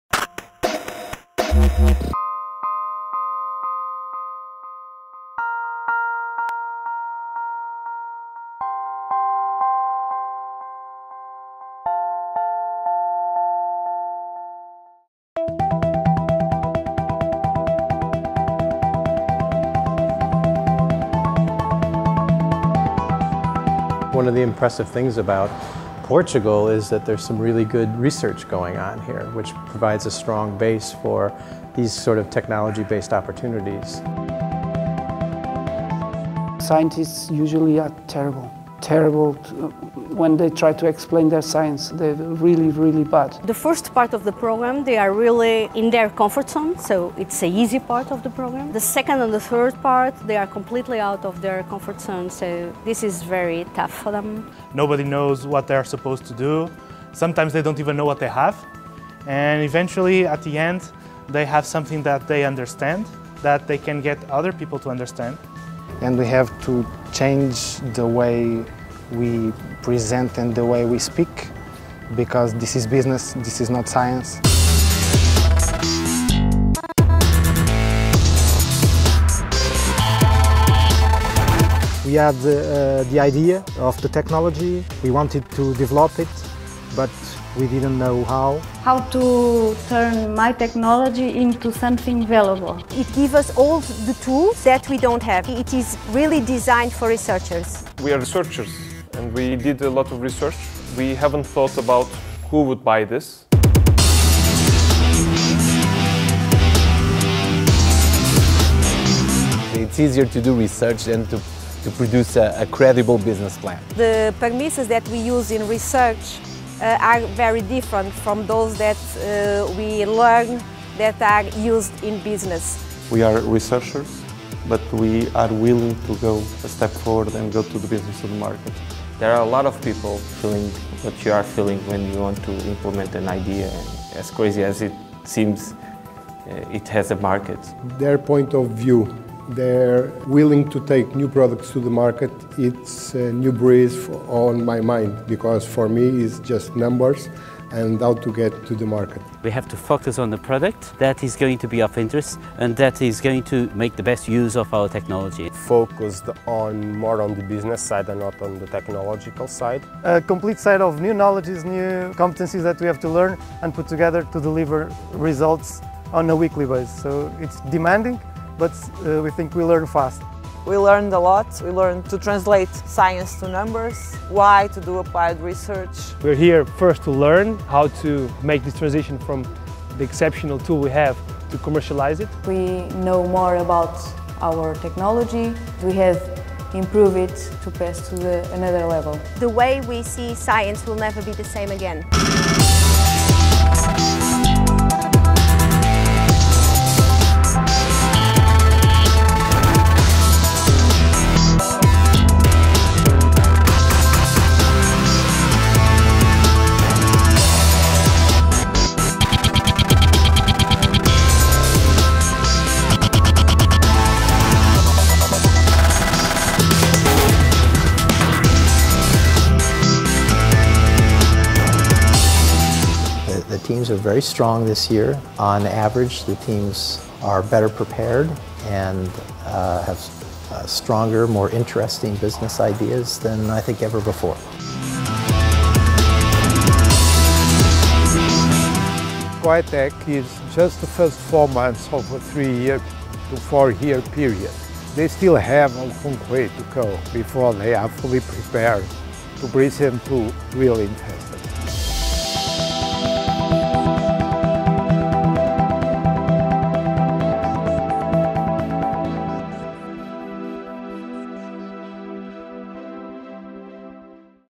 COHiTEC 2013 Teaser
Teaser about the 2013 edition of COHiTEC, a training program in new technology evaluation.